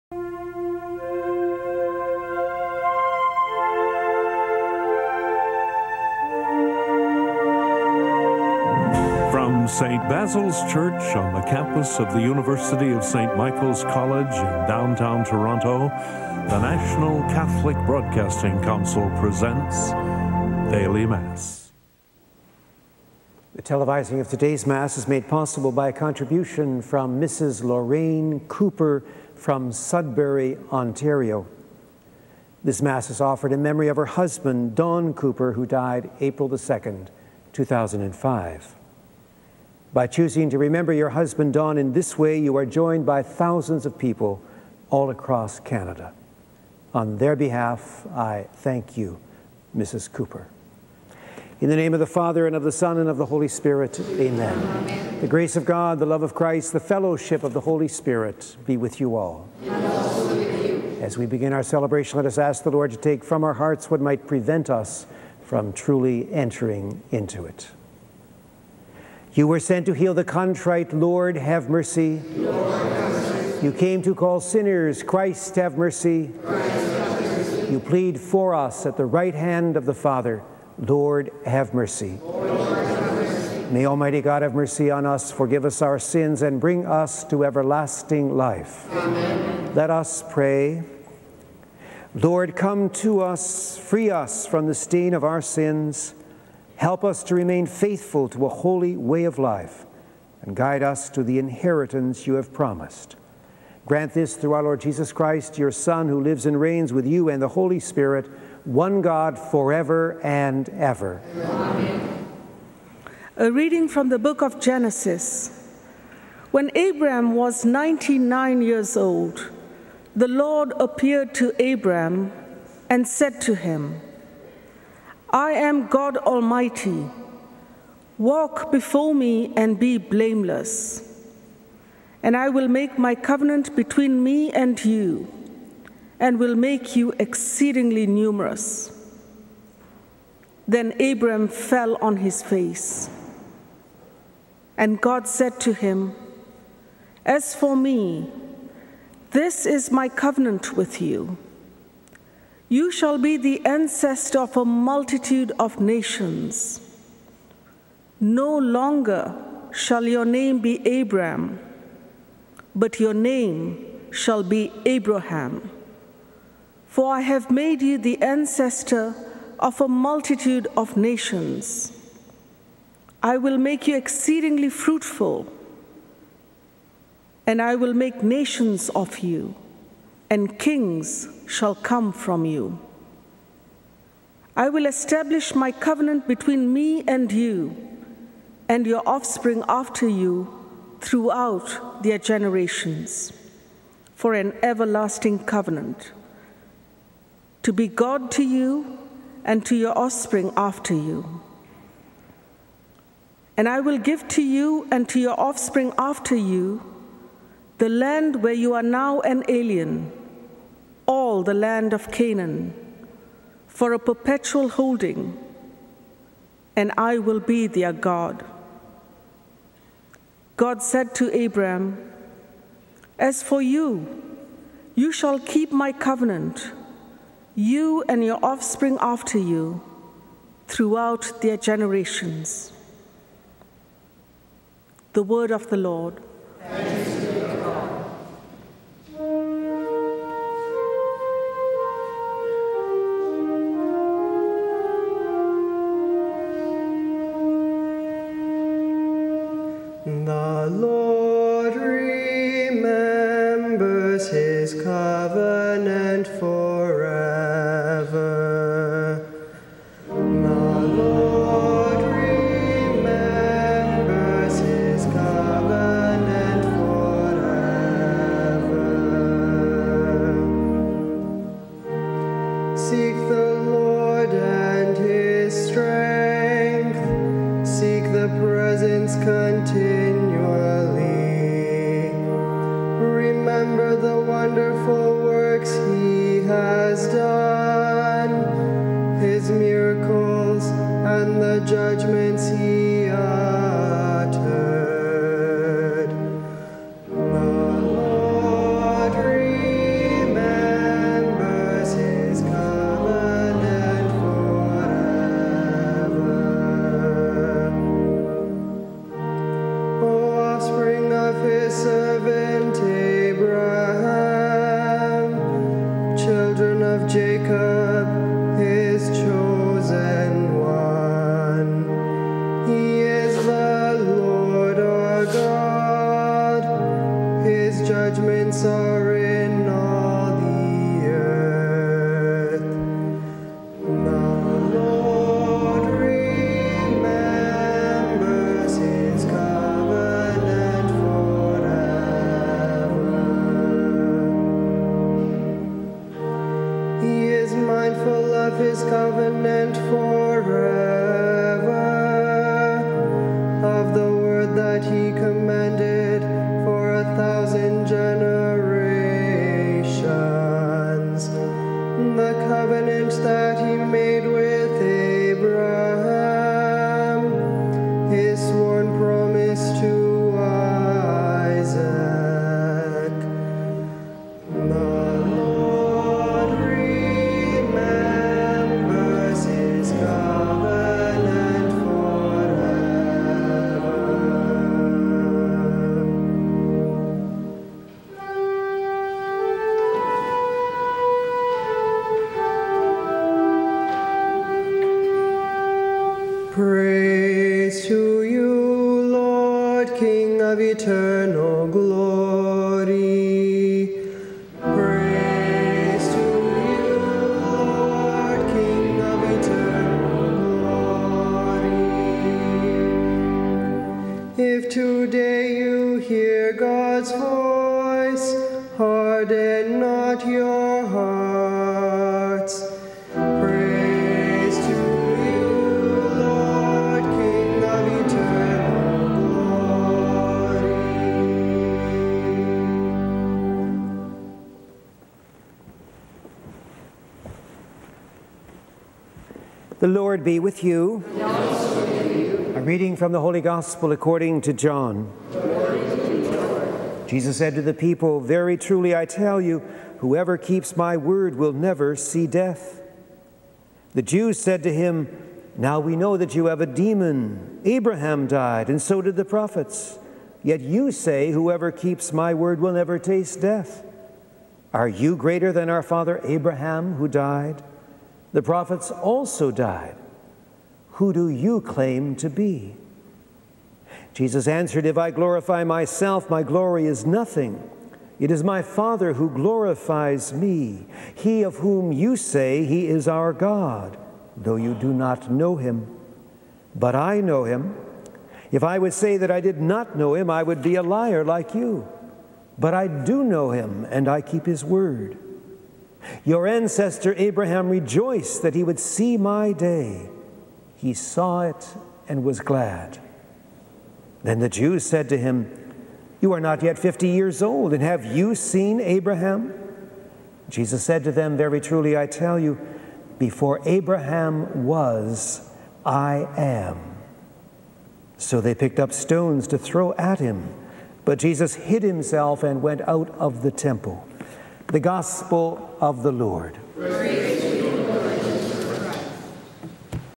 Daily Mass 1/3 Thursday 02 April 2009
Fr. Donovan celebrates the entire Daily Mass in 3 parts, from the English-speaking Catholic Bishops of Canada and St. Basil's Church in Toronto.